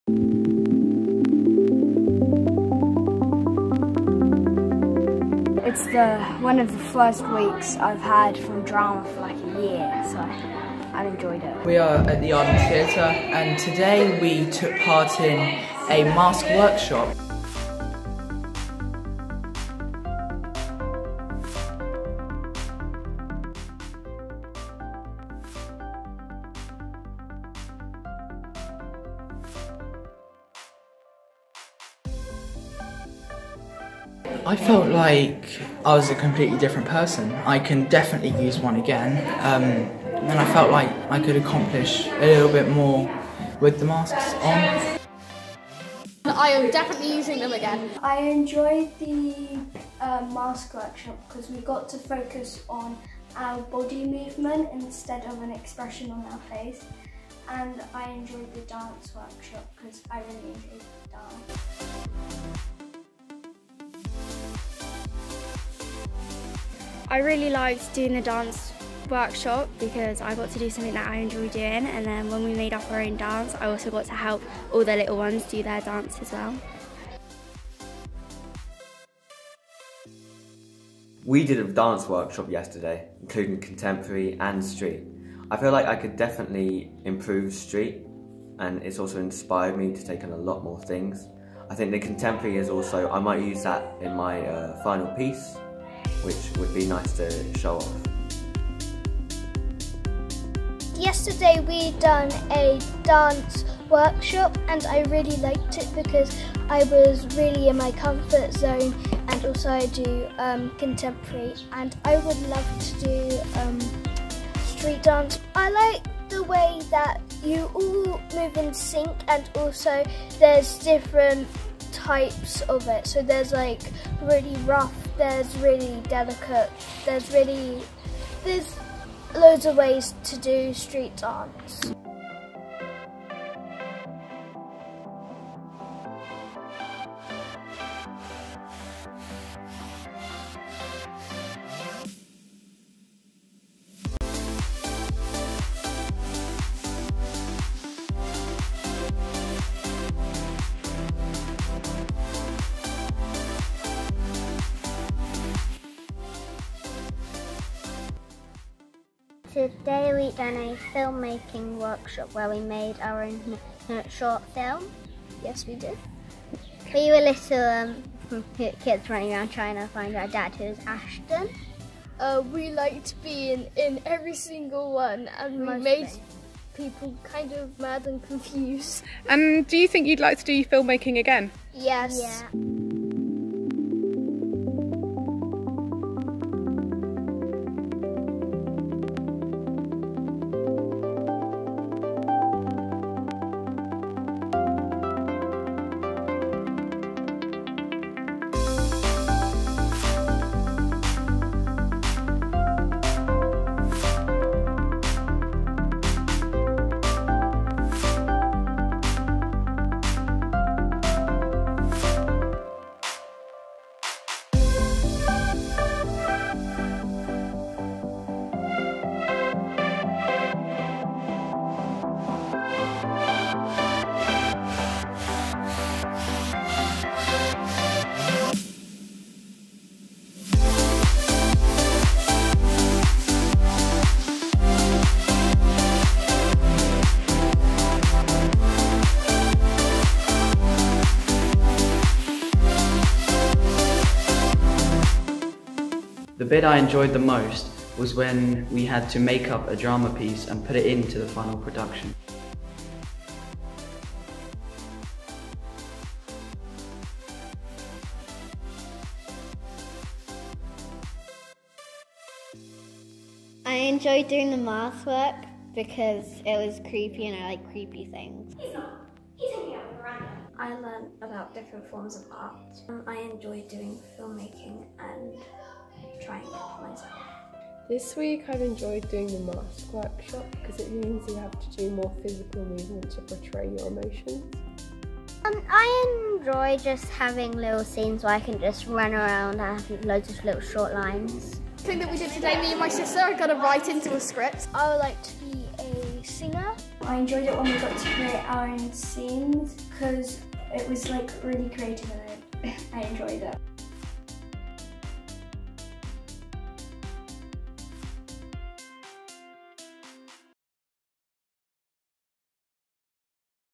Amazing Arts Award Week 2022
This summer, Vibe had an excellent time, partnering with The Arden Theatre Faversham for an Arts Award week! We loved exploring the theatre, the types of art & roles people have there, and taking part in workshops!  For our final performance, we worked really hard and put together a retelling of scenes from The Tempest, using our newfound skills! Some of us had never been on stage before! We had an awesome time and we hope you did too!